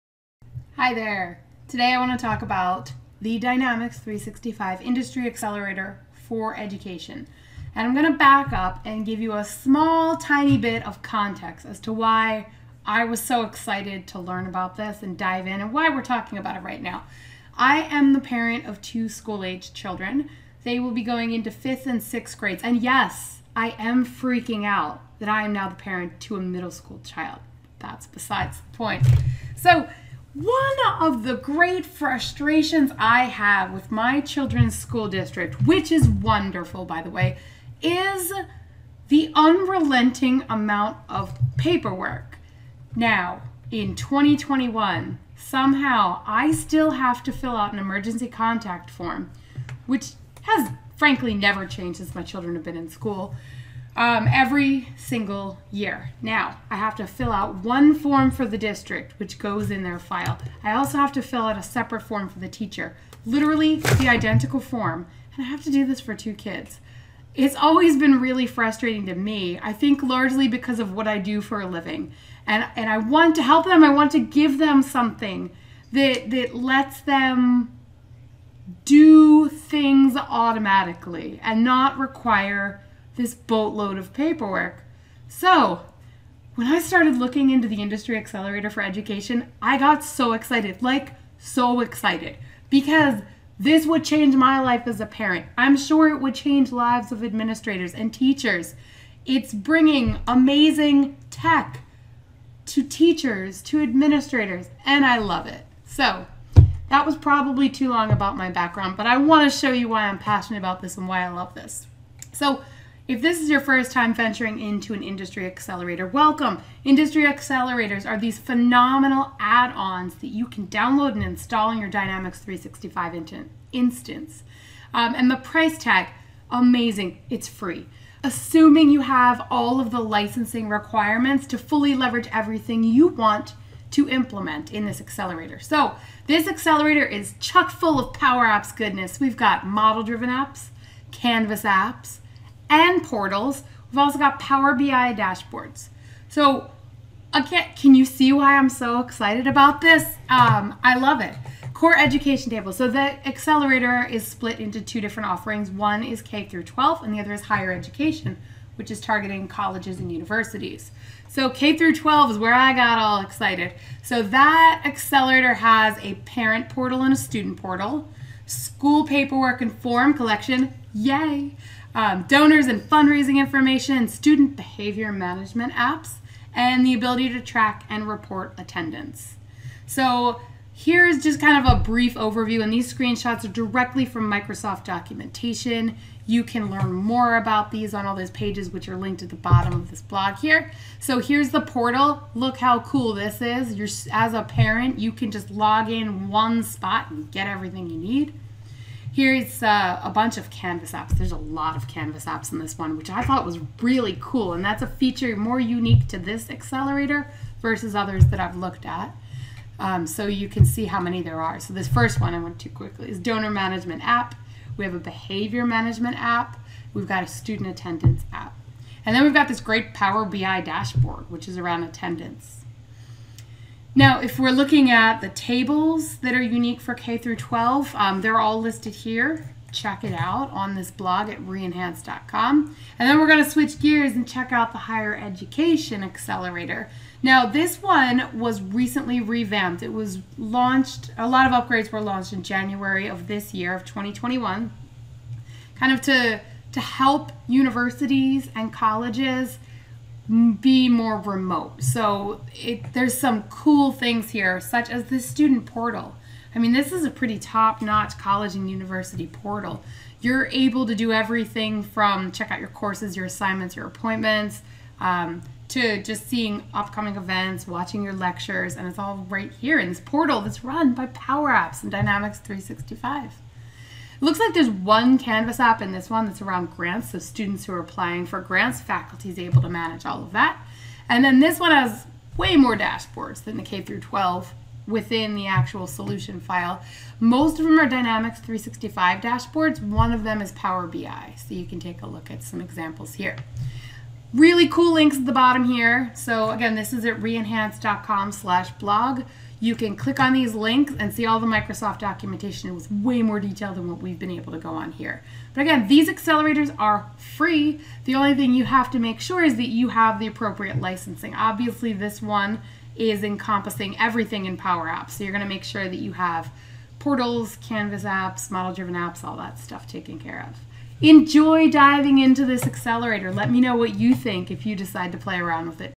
Review & Overview of the Dynamics 365 Education Industry Accelerator
I LOVE this industry accelerate for Dynamics 365! Check out the high-level features available - including model-driven apps, canvas apps, portals, dashboards and more! Here's a link to the blog that accompanies this video review

0:00 Intro & Why I care about this
1:52 Industry Accelerator Overview
2:15 Education Accelerator
2:42 K-12
3:53 Higher Education
4:54 Resources